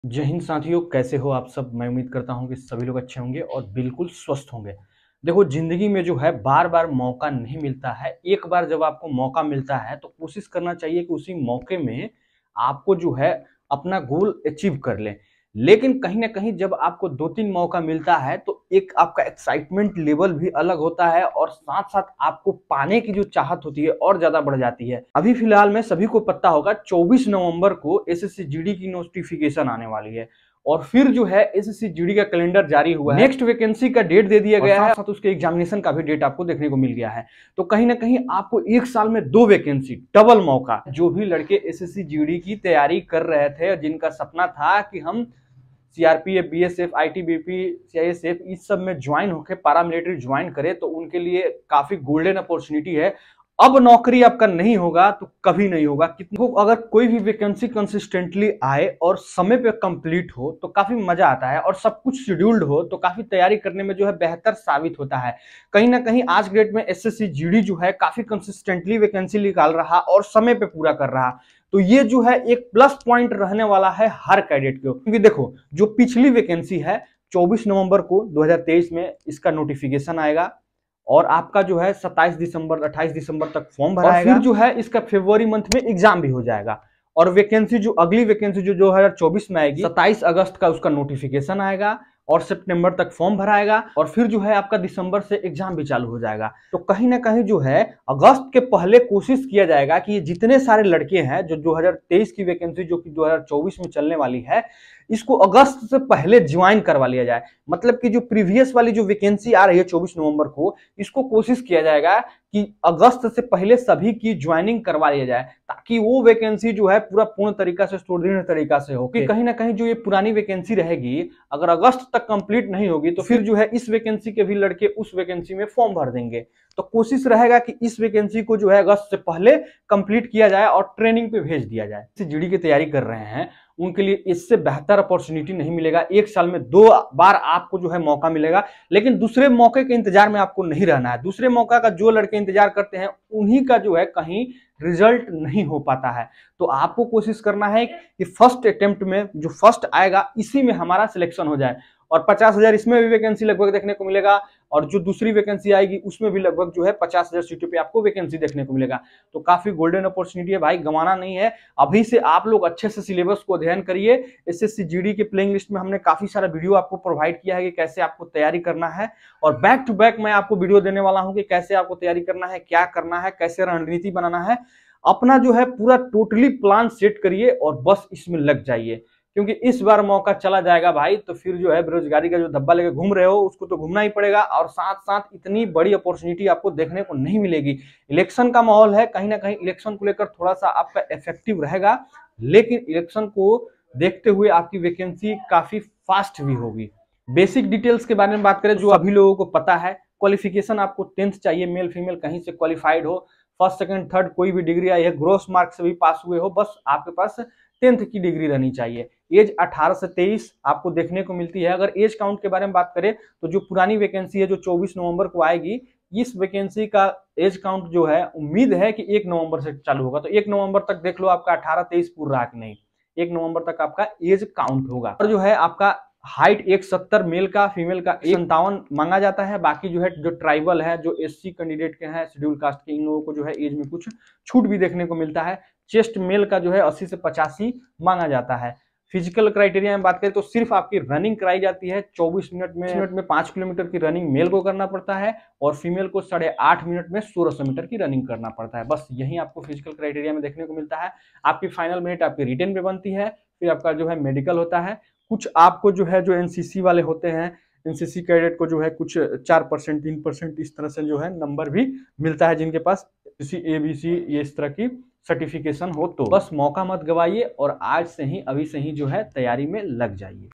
ssc gd 2023-24 एक साल में दो भर्ती✅ 01 लाख तक Post होंगे! Age कब से Count होगा😱
ssc gd 2023-24 एक साल में दो भर्ती✅ 01 लाख तक Post होंगे!
Ssc gd
Ssc gd exam dates
Ssc gd Bharti